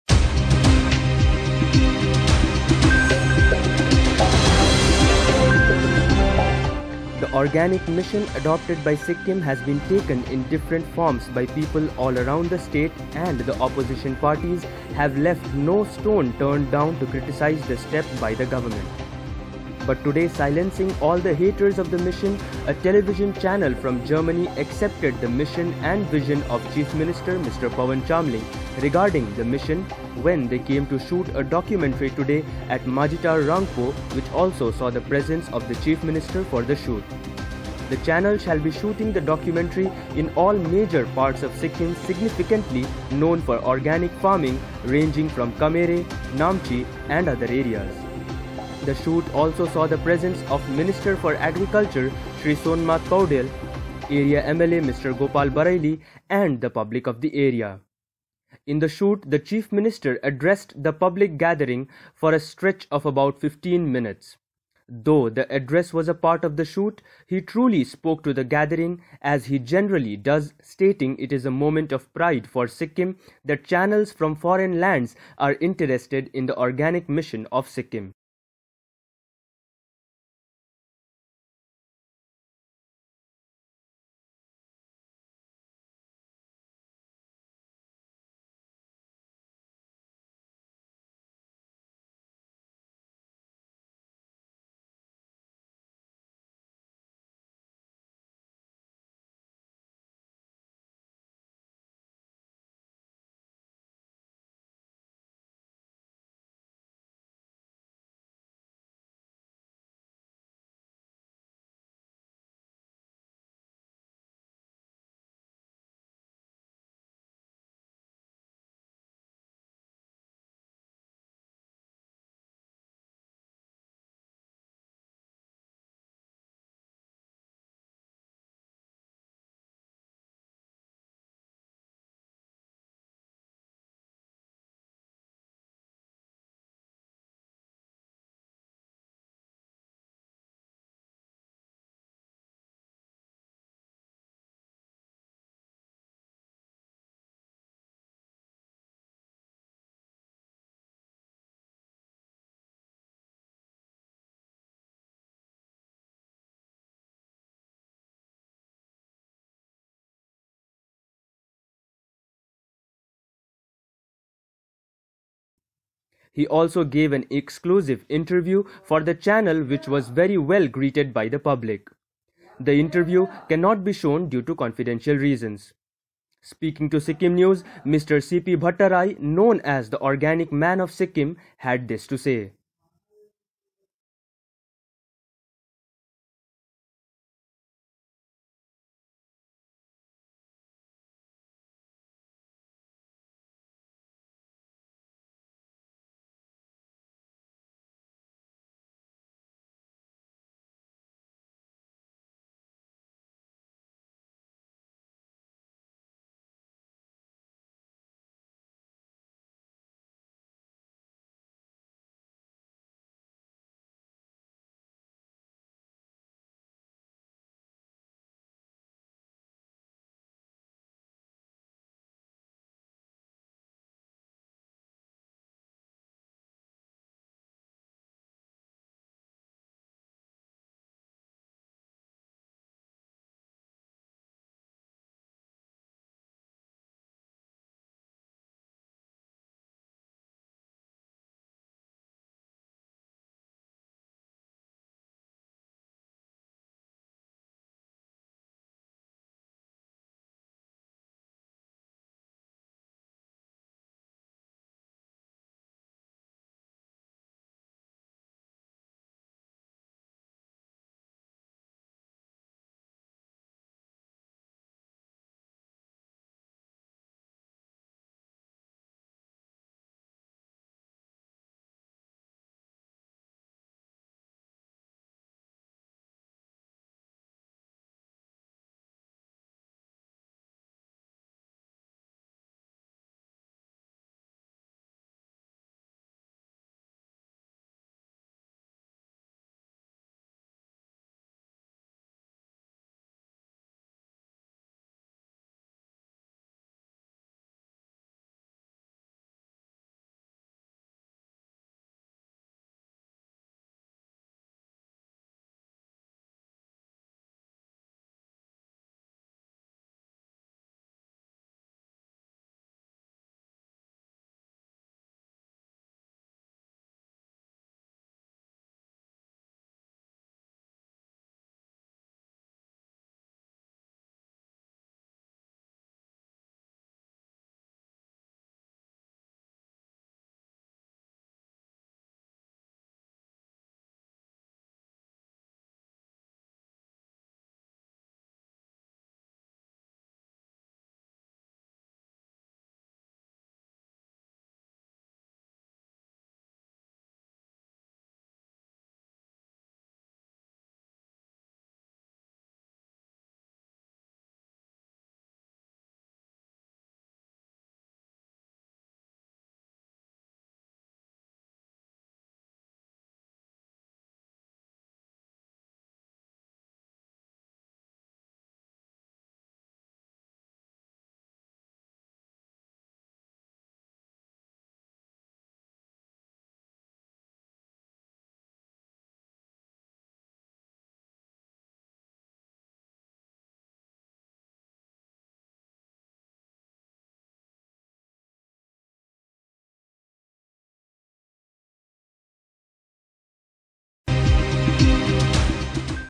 Organic Sikkim in Germany |Shooting|Documentary|Pawan Chamling|Majhitar
The organic mission adopted by Sikkim has been taken in different forms by people all around the state and the opposition parties have left no stone turned down to criticize the step.

But today silencing all the haters of the mission, a television channel from Germany accepted the mission and vision of Hon'ble Chief Minister, Mr. Pawan Chamling regarding the mission when they came to shoot the documentary today at Majhitar, Rangpo which also saw the presence of the Chief Minister for the shoot.

The channel shall be shooting the documentary in all the major parts of Sikkim significantly known for Organic Farming ranging from Kamerey, Namchi and other areas.

The shoot also saw the presence of Minister for Agriculture, Shri Somnath Poudyal, Area MLA, Mr. Gopal Baraily, and the public of the area.

In the shoot, the Chief Minister, addressed the public gathering for a stretch of about 15 minutes. Though the address was a part of the shoot, he truly spoke to the gathering as he generally does stating it is a moment of pride for Sikkim that channels from foreign lands are interested in the Organic Mission of Sikkim. He also gave an exclusive interview for the channel which was very well greeted by the public.

Organic Man of Sikkim, Mr. C P Bhattarai also spoke to the media during the occasion.